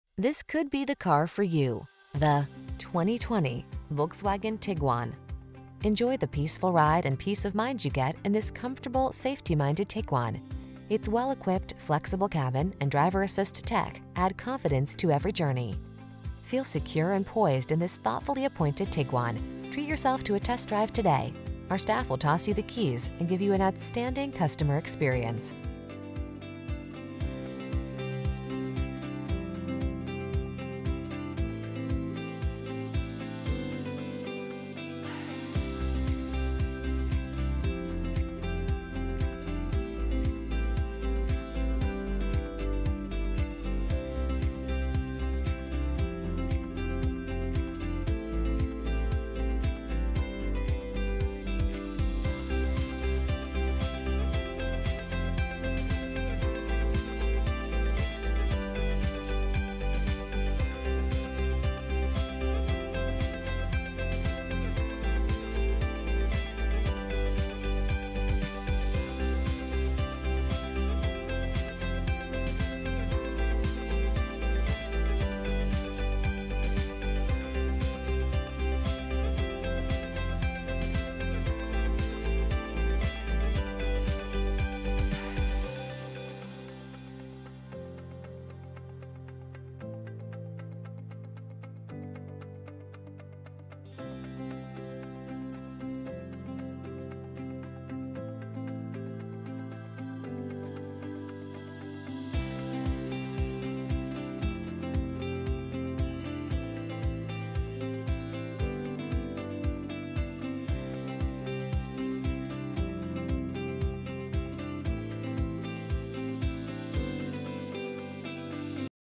2020 Volkswagen Tiguan Las Vegas, Henderson, North Las Vegas, Boulder City, Mesquite, NV P021795A
Platinum Gray Metallic Used 2020 Volkswagen Tiguan  available in Las Vegas, Nevada at Findlay North Volkswagen. Servicing the Henderson, North Las Vegas, Boulder City, Mesquite, NV area.

Recent Arrival!brbrCERTIFIED Warranty good through X of X, or 84,000 miles. 100+ point inspection performed by a qualified Volkswagen technician, then reconditioned to strict standards using Volkswagen genuine parts.brbrbrThis Tiguan is equipped with the following features:brbrOdometer is 11555 miles below market average! 22/29 City/Highway MPG